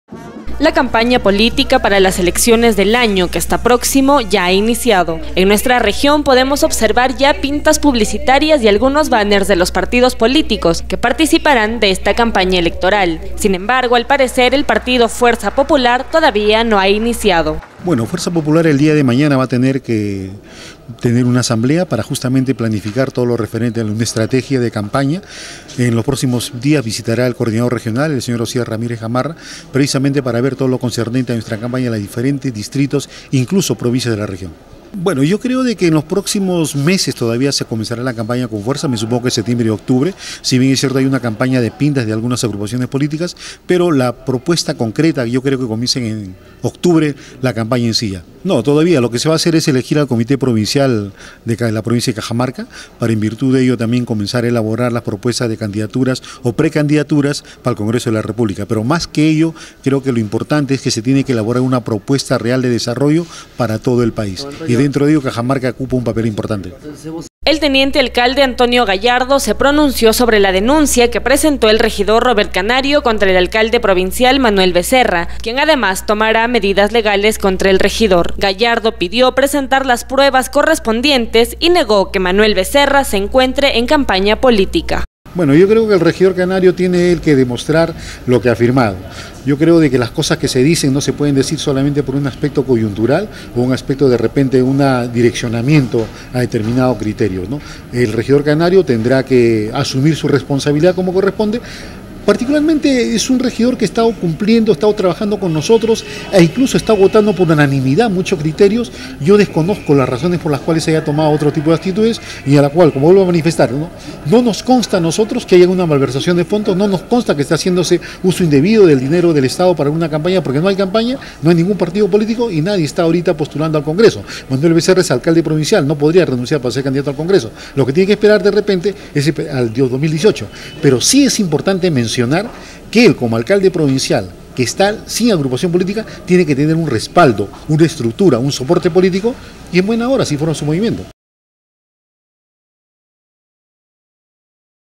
Fuerza Popular iniciará campaña
La campaña política para las elecciones del año que está próximo ya ha iniciado, en nuestra región podemos observar ya pintas publicitarias y algunos banners de los partidos políticos que participarán de esta campaña electoral, sin embargo al parecer el partido Fuerza Popular todavía no ha iniciado.
Asimismo el teniente alcalde, Antonio Gallardo, se pronunció sobre la denuncia que presentó el regidor Robert Canario contra el alcalde provincial Manuel Becerra, quien además tomará medidas legales contra el regidor, gallardo pidió presentar las pruebas correspondientes y negó que Manuel Becerra se encuentre en campaña política.